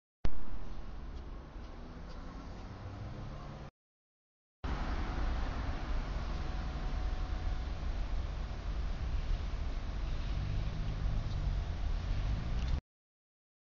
[Szczecin] Wypadek na budowie CH Kaskada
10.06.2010. Wypadek na placu budowy. Przewrócił się żuraw samojezdny TEREX/DEMAG AC 80-2